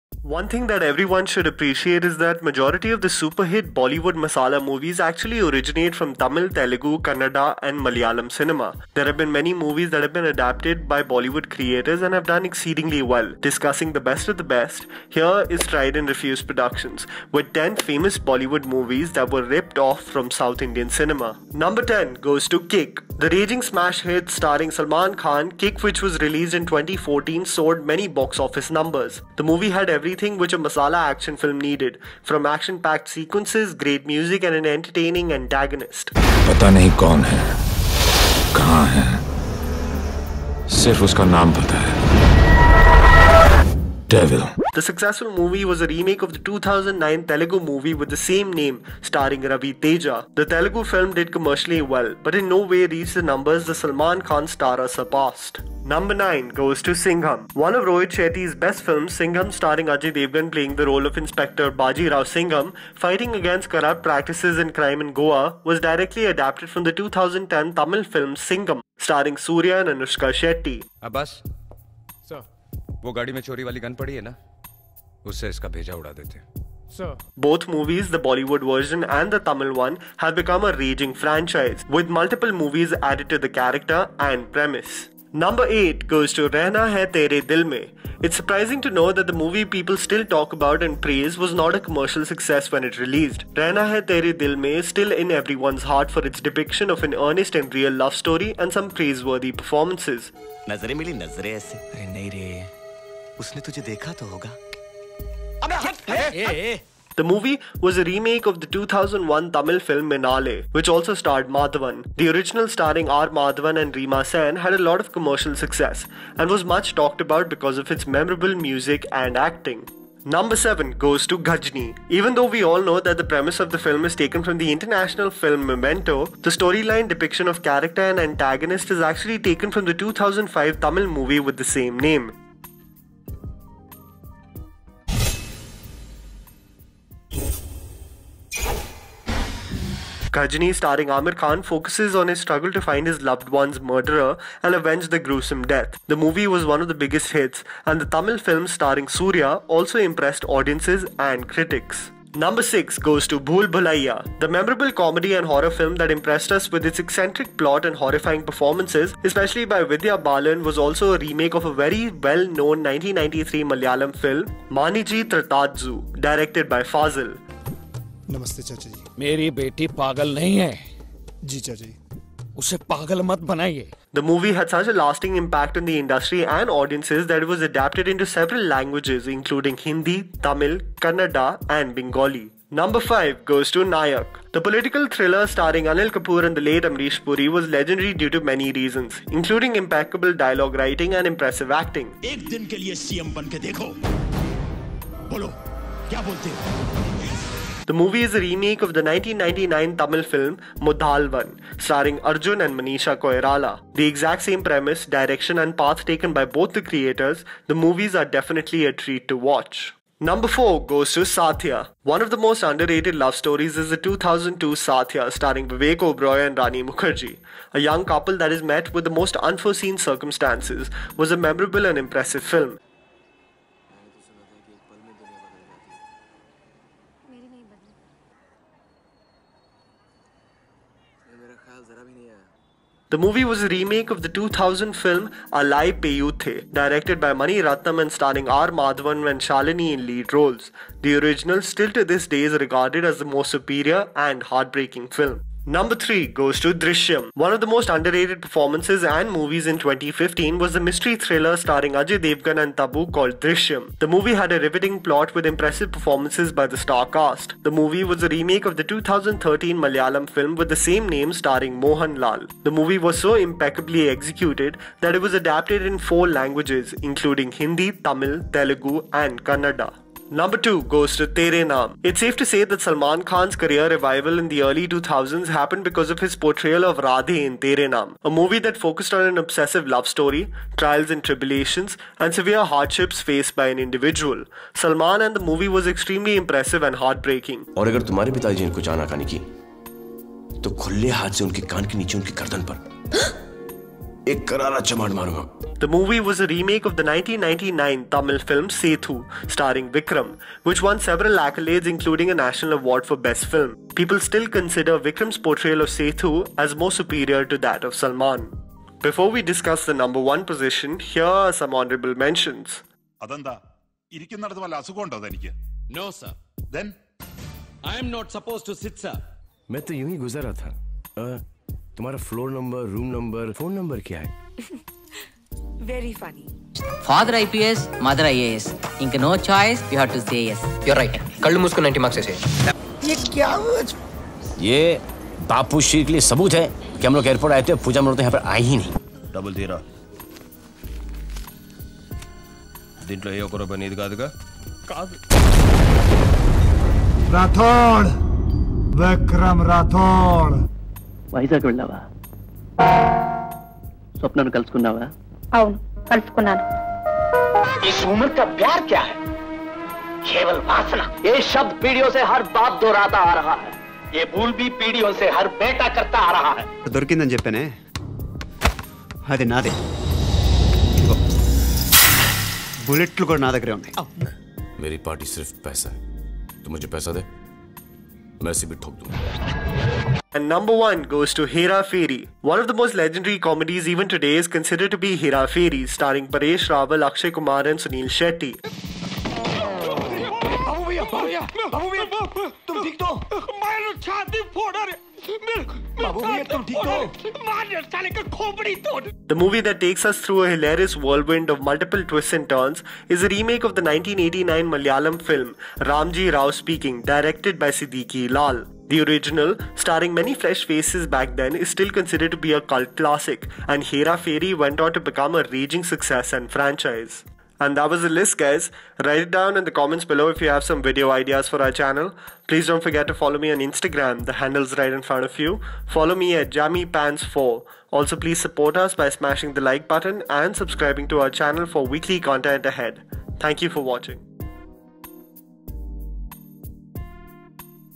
10 Famous Bollywood Movies That Were Ripped Off From South Indian Cinema
What did you think of the list?